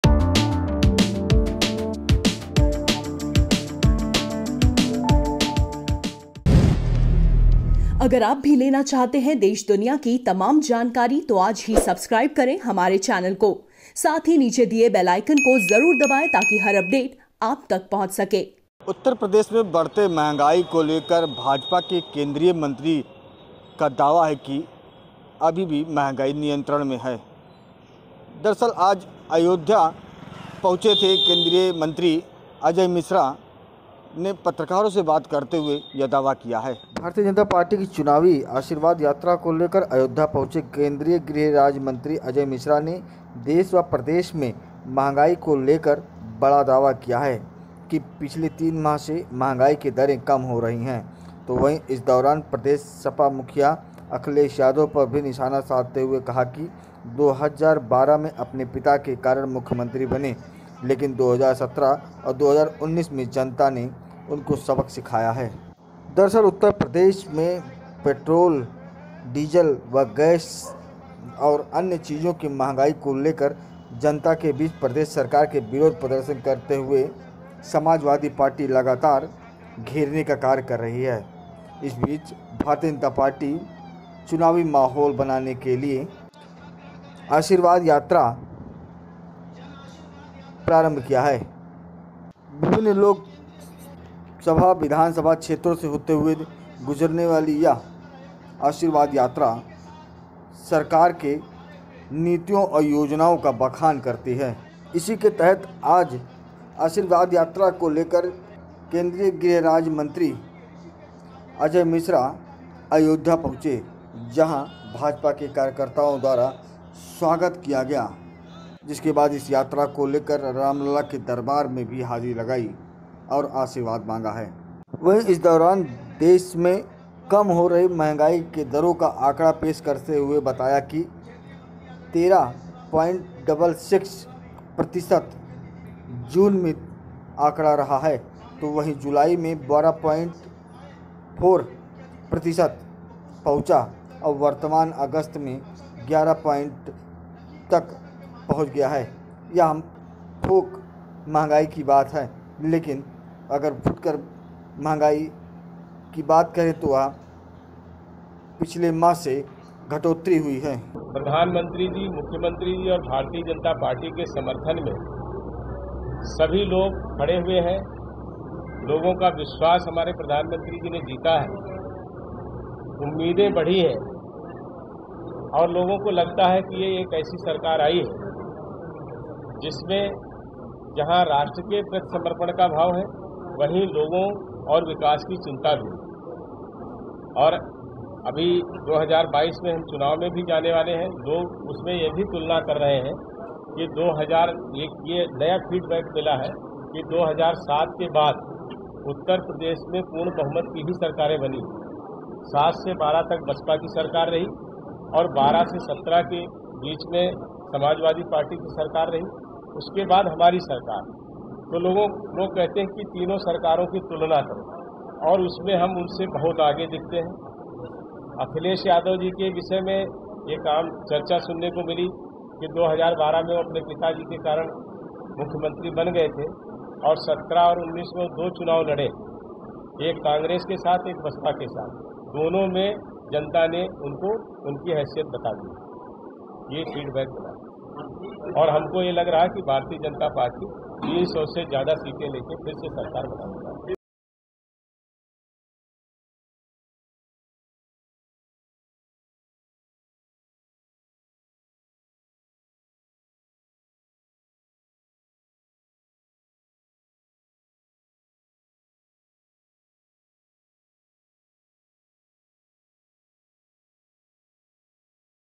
Jan Ashirwad Yatra को लेकर अयोध्या पहुंचे केंद्रीय गृह राज्य मंत्री अजय मिश्रा। BJP Uttar Pradesh
भारतीय जनता पार्टी की जन आशीर्वाद यात्रा को लेकर अयोध्या पहुंचे केंद्रीय गृह राज्य मंत्री अजय मिश्रा का सरयू घाट के पास सैकड़ों की संख्या में मौजूद कार्यकर्ताओं ने स्वागत किया,  जिसके बाद इस यात्रा को लेकर श्री रामलला के दरबार भी पहुंचे। दर्शन पूजन के बाद मीडिया से मुखातिब होते हुए केंद्रीय गृह राज्य मंत्री ने सरकार के योजनाओं का बखान किया तो वही देश में घटते महंगाई की दरों का आंकड़ा भी सामने रखा। दादा ने दावा किया कि उत्तर प्रदेश में भाजपा की सरकार बनने जा रही है। वहीं इस दौरान प्रदेश सपा मुखिया अखिलेश यादव पर भी निशाना साधा और कहा कि 2012 में अपने पिता के कारण मुख्यमंत्री बने लेकिन 2017 व 19 में जनता ने सबक सिखाया है।

00:00  INTRO
00:37  भाजपा की जन आशीर्वाद यात्रा
 02:41 अजय मिश्रा